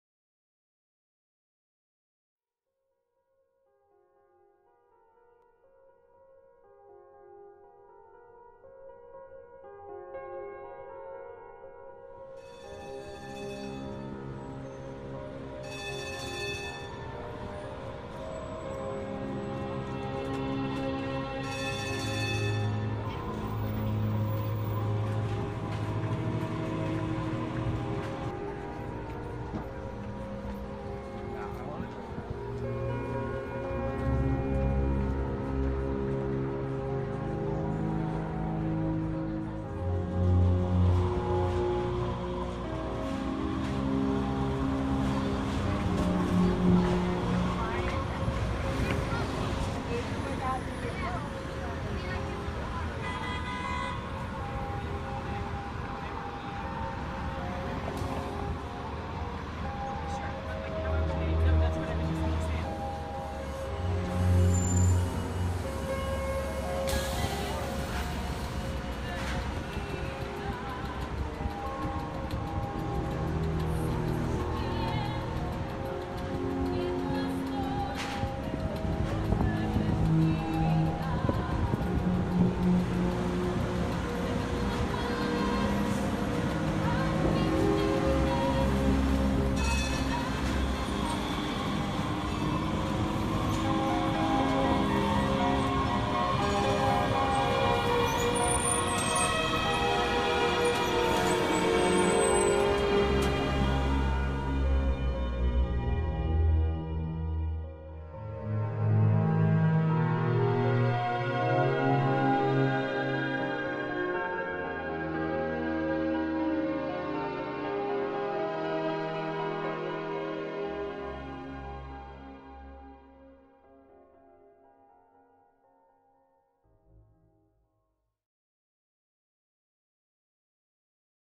Irving
A short visual collage of a city corner in San Francisco, created in Photoshop CC to demonstrate some of the video editing capabilities of that program for Episode 39 of TheFIX. All video editing done in Photoshop, Sound editing done in Adobe Audition.